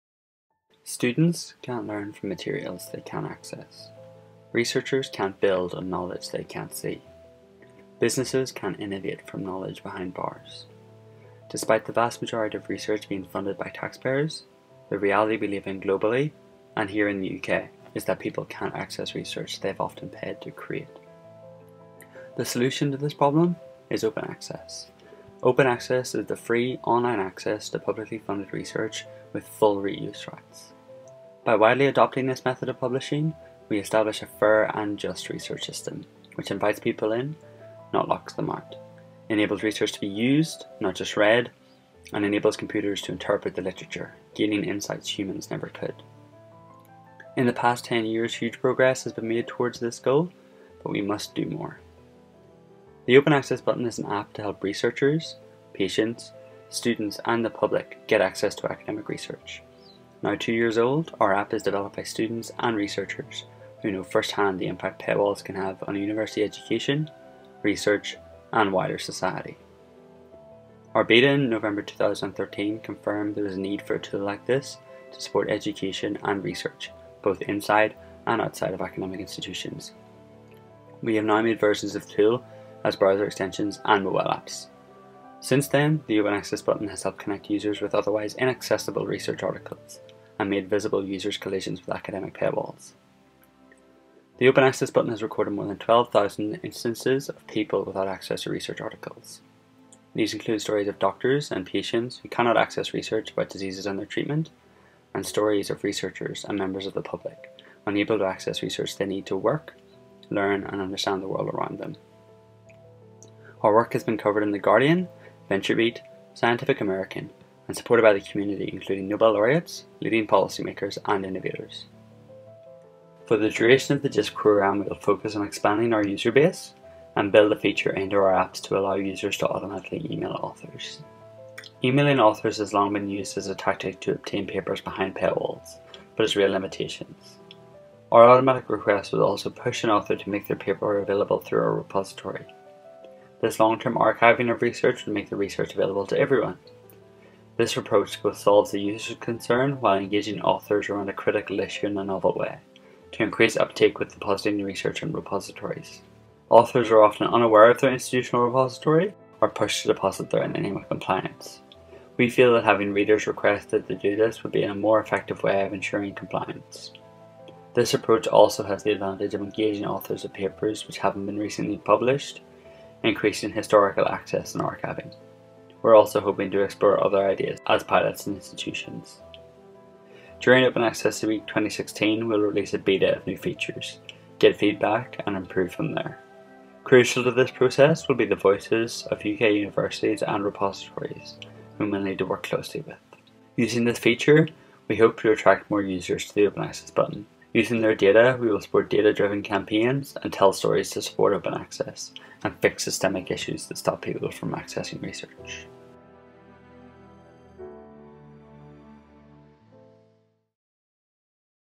Open Access Button JISC 2015 Video
A video we put together for JISC start up competition. You can vote for us in that competition